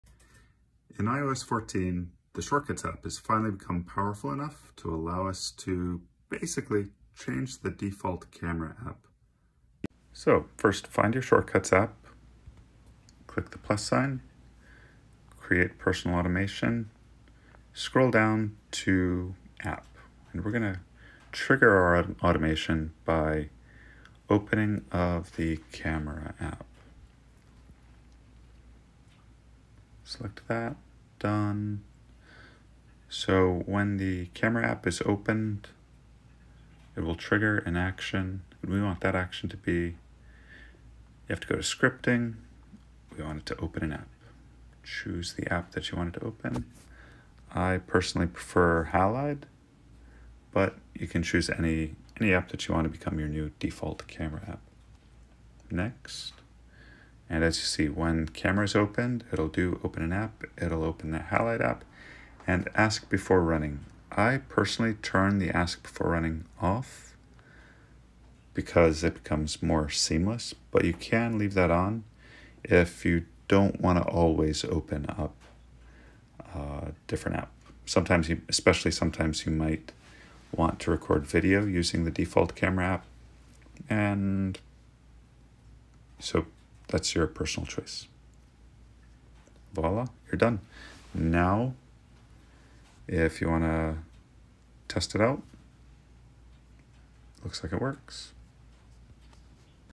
Change Default Camera App in iOS
Quick instructions on how to select a different default Camera App in iOS 14 by creating an automation in the Shortcuts app.